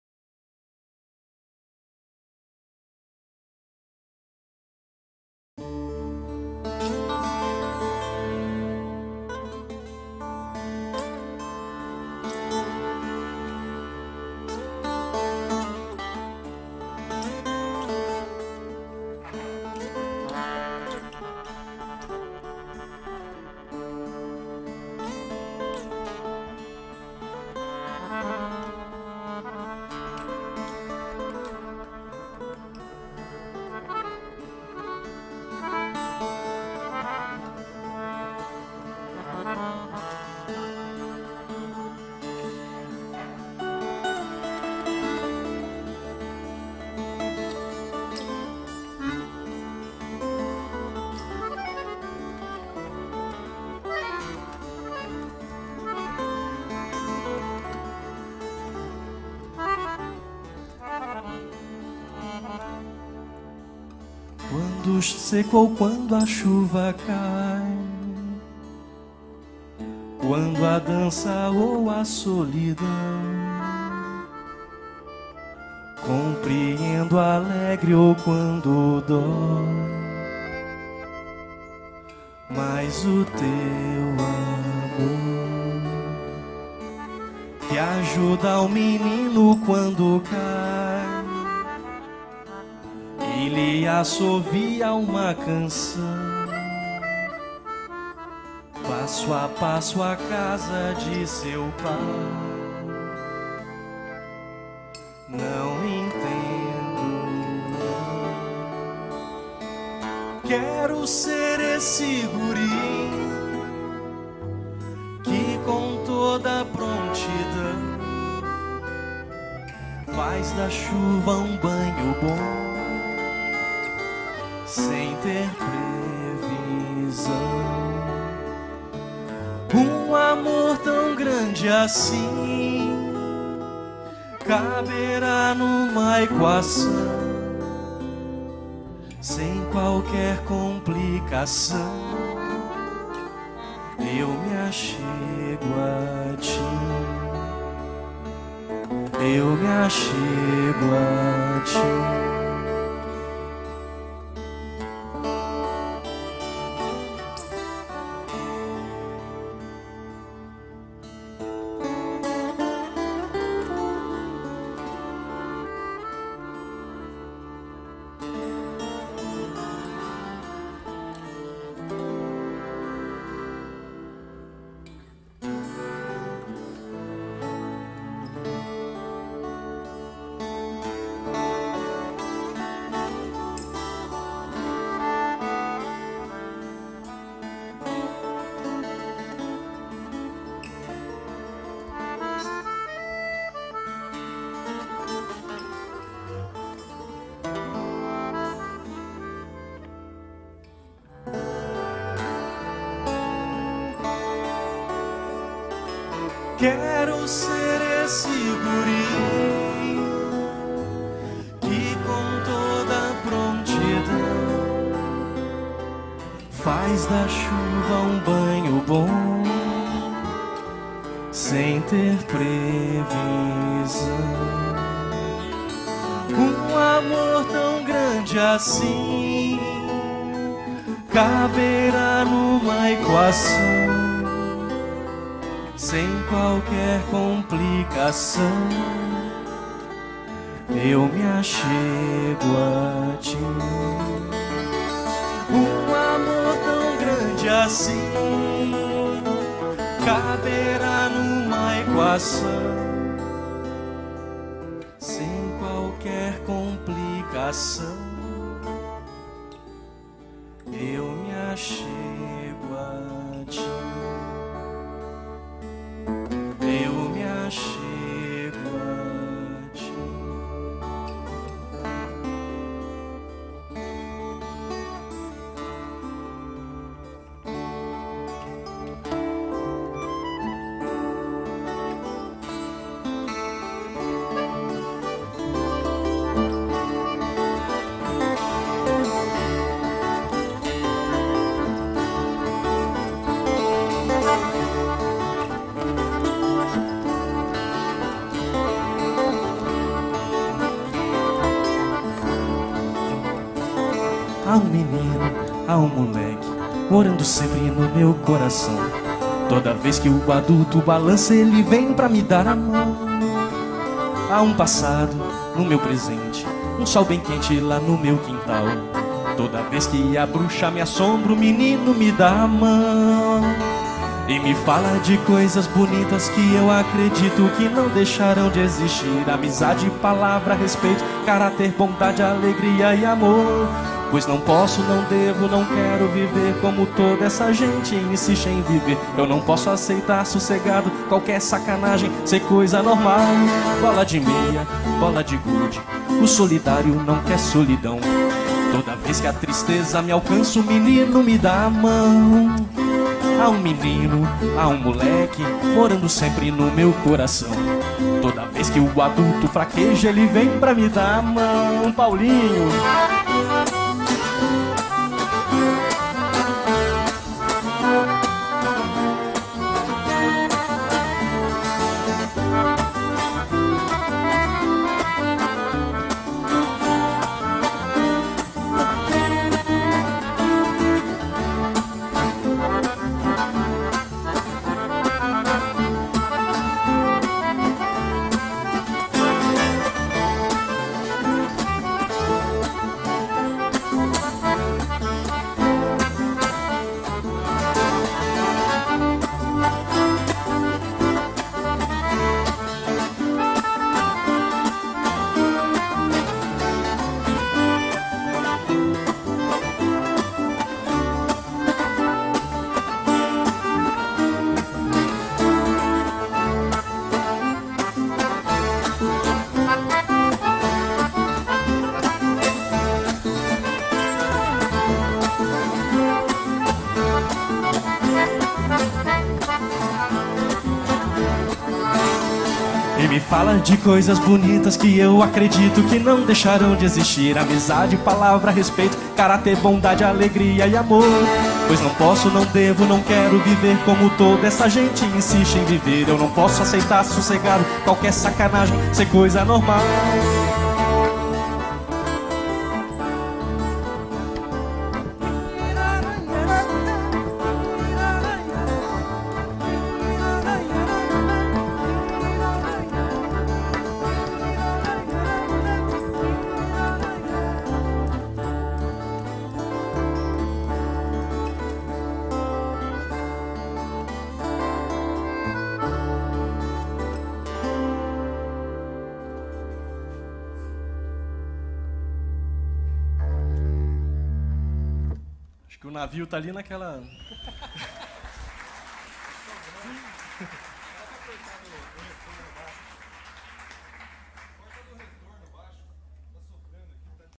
Arlindo Lima: NMB 7 / 2013 - Lifestream
Registro feito no Nossa Música Brasileira 7 / 2013 pela Lifestream Vídeos.
Com a autorização deles, reproduzo parcialmente o material. Aqueles que desejarem o material completo, favor entrar em contato com Lifepictures/Lifestream.